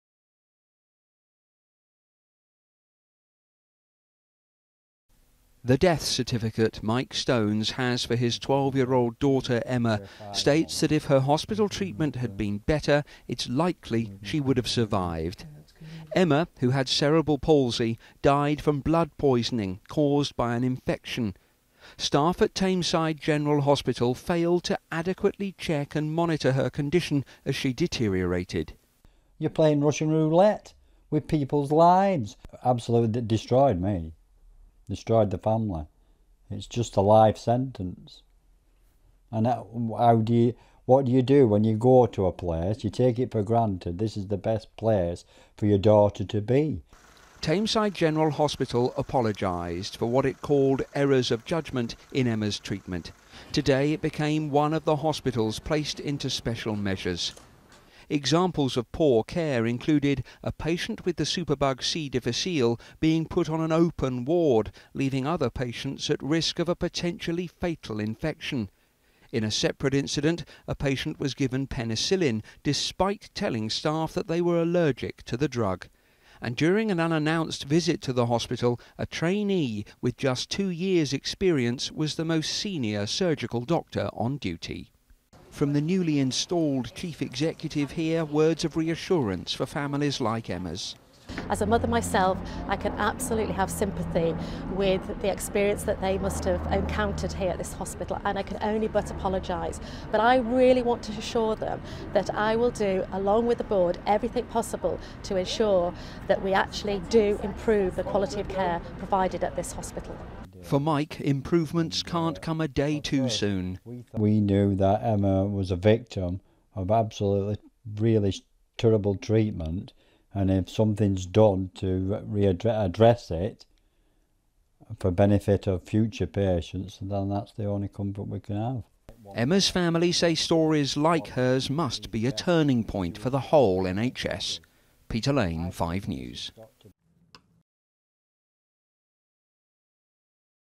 Tameside hospital comes under fire
Peter Lane has been speaking to a father whose daughter suffered from the hospital's mistakes.